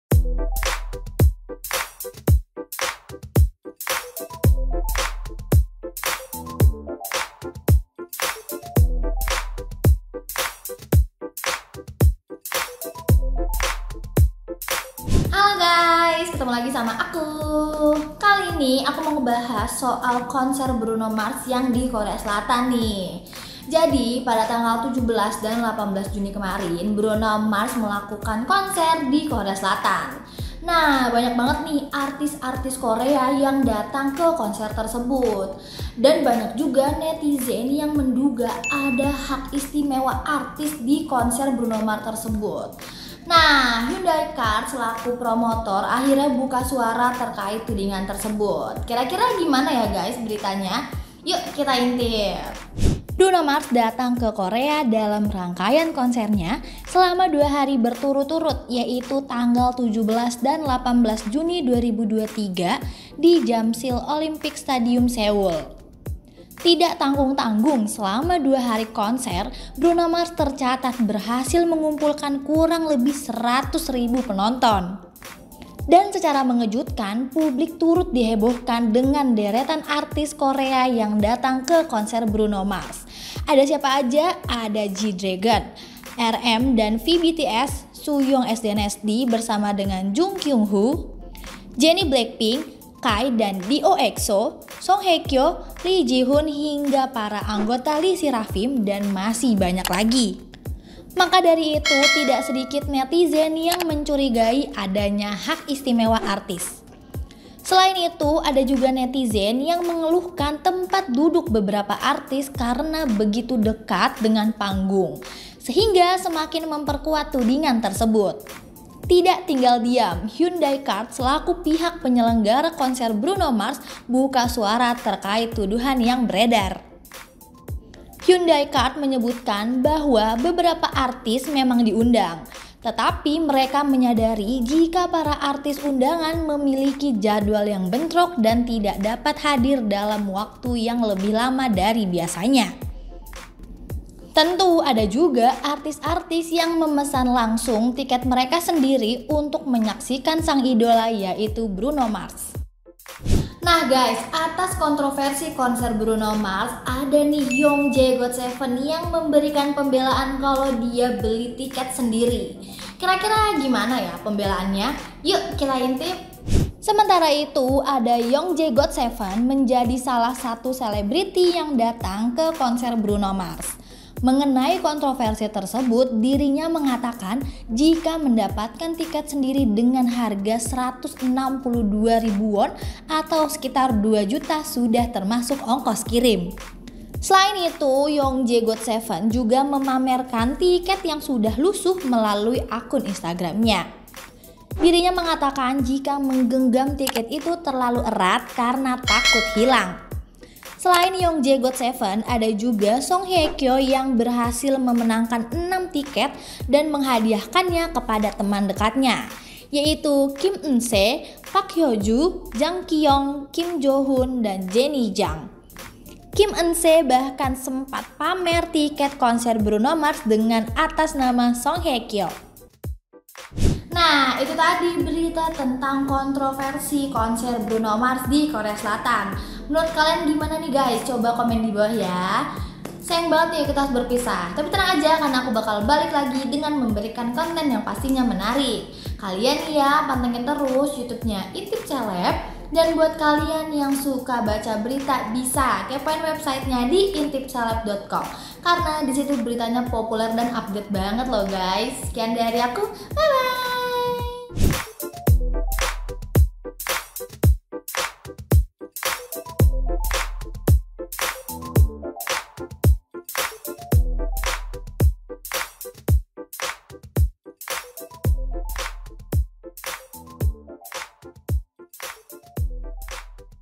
Kontroversi Banyak Seleb ke Konser Bruno Mars, Youngjae GOT7 Pamer Beli Tiket Sendiri | INSEL Update
IntipSeleb -  Bruno Mars baru saja menjalankan konser di Korea Selatan 17-18 Juni 2023 di Jamsil Olympic Stadium, Seoul. Konser tersebut dihadiri oleh banyak kalangan selebriti papan atas Korea Selatan.

Atas banyaknya kontroversi, Youngjae GOT7 pun beri pembelaan jika diirnya beli tiket sendiri.